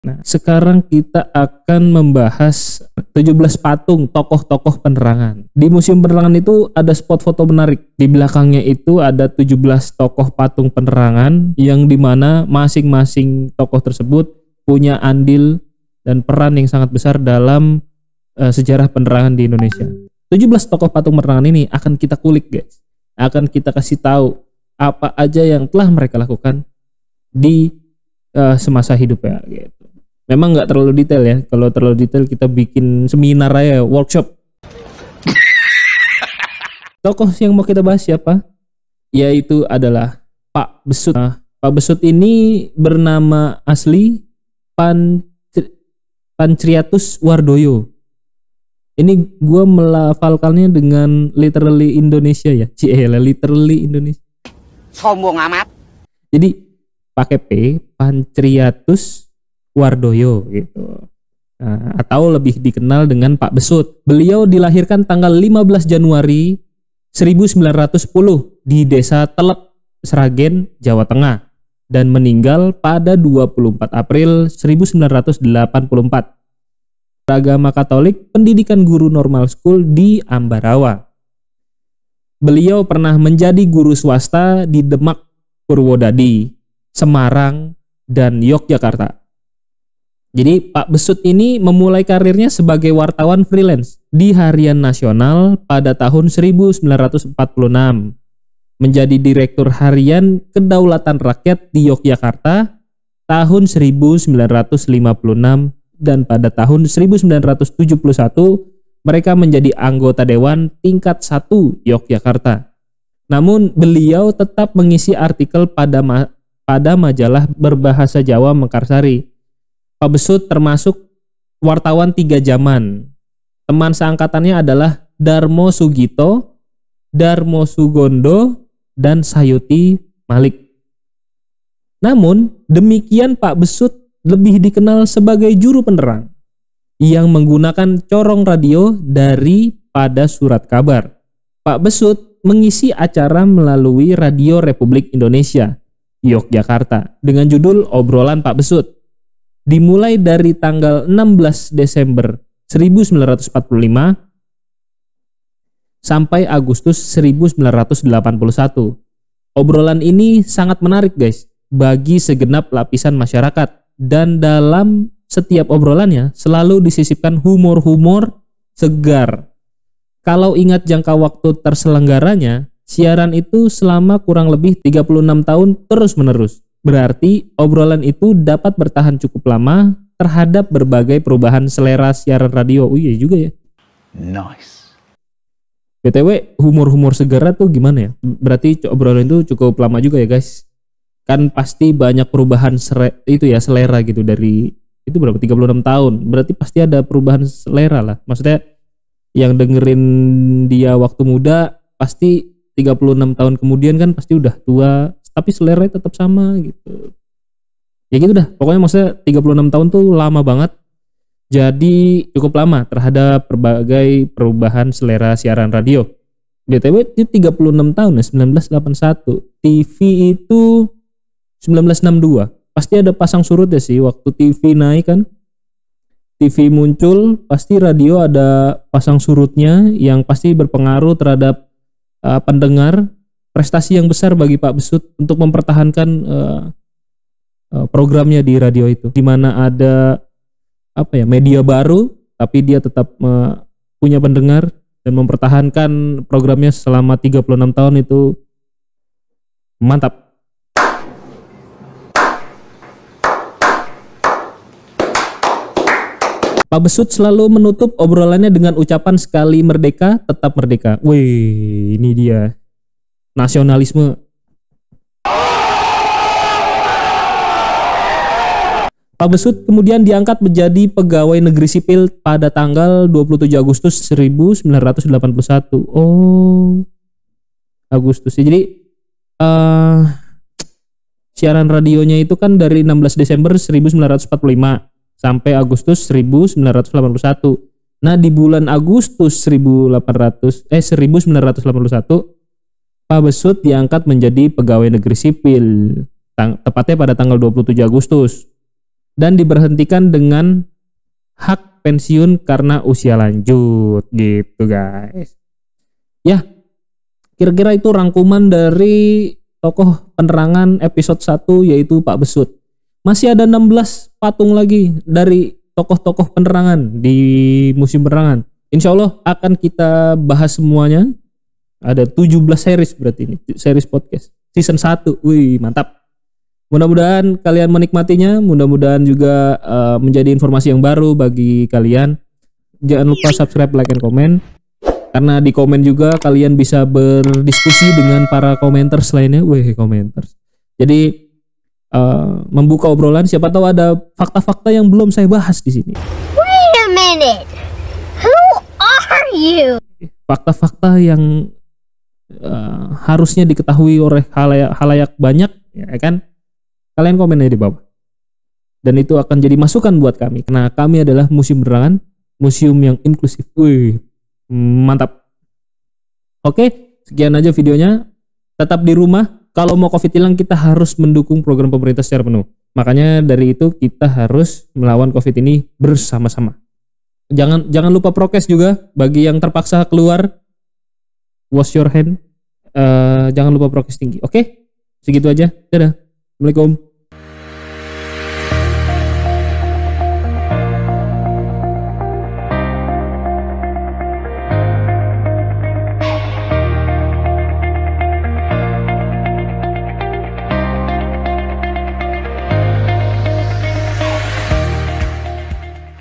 Siapakah Pak Besut? Tokoh-Tokoh Penerangan #1
Pak Besut

Tokoh ini bernama Pancratius Wardoyo atau lebih dikenal dengan Pak besut. Beliau dilahirkan tanggal 15 januari 1910 di desa Telep, Sragen, Jawa Tengah. Dan meninggal pada tanggal 24 April 1984 beragama katolik pendidikan guru normal school di ambarawa. Beliau pernah menjadi guru swasta di demak, purwodadi, semarang dan yogyakarta. Pak besut memulai karirnya sebagai wartawan freelance di harian nasional pada tahun 1946, menjadi redaktur harian kedaulatan rakyat Yogyakarta tahun 1956 dan pada tahun 1971 menjadi anggota dewan tingkat satu yogyakarta. Namun beliau tetap mengisi artikel pada majalah berbahasa jawa mekarsari, pak besut termasuk wartawan tiga jaman teman seangkatannya adalah darmosoegito, darmosugondo dan sayuti malik, namun demikian pak besut lebih dikenal sebagai juru penerang yang menggunakan corong radio dari pada surat kabar. Pak besut mengisi acara melalui radio republik Indonesia Yogyakarta dengan judul obrolan pak besut dimulai dari tanggal 16 desember 1945 sampai agustus 1981, obroln ini sangat menarik bagi segenap lapisan masyarakat dan dalam setiap obrolannya selalu disisipkan humor-humor segar kalau ingat jangka waktu terselenggaranya siaran itu selama kurang lebih 36 tahun terus menerus berarti obrolan itu dapat bertahan cukup lama terhadap berbagai perubahan selera siaran radio serta pasang surutnya siaran radio karena munculnya televise ini merupakan prestasi yang besar. Pak besut selalu menutup obrolannya dengan ucapan sekali merdeka tetao merdeka, pak besut kemudian diangkat jadi pegawai negeri sipil tanggal 27 agustus 1981 dan diberhentikan dengan hak pensiun karena usia lanjut